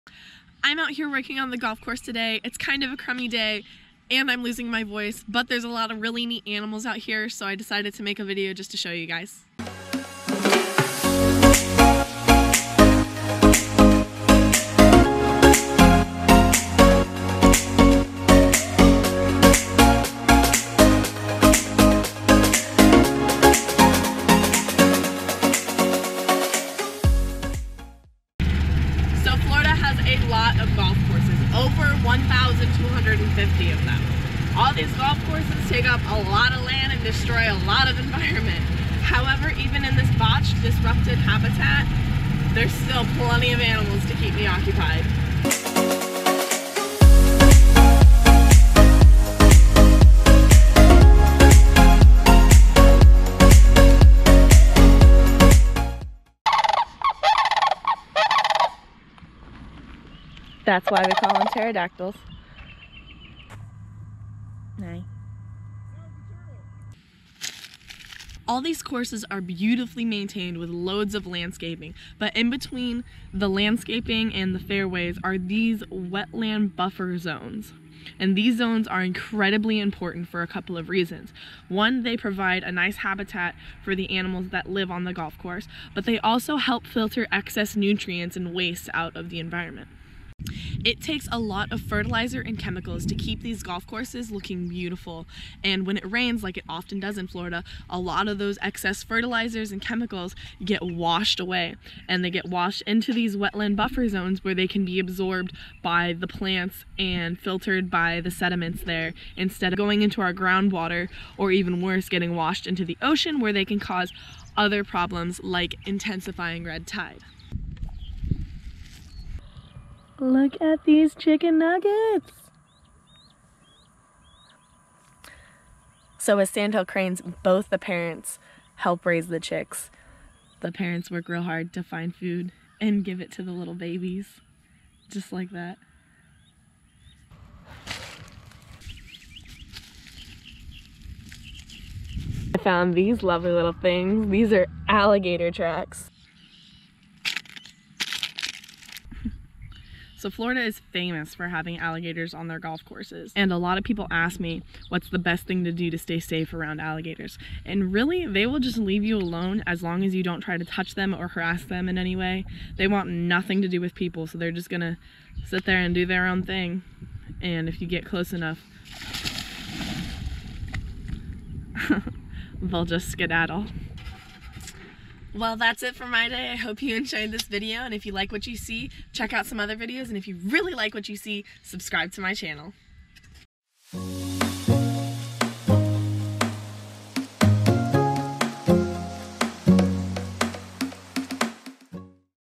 Golf Course Habitat
I am a biology student and I love wildlife. When I work on the golf course I love to see and observe all of the wildlife in the area. In the video I talk about wetland buffer zones, these areas are critical for the ecosystem to make sure excess fertilizers aren't washed to other habitats. I also talk about and showcase some really awesome creatures

If you want to know more about alligator safety check out this video!!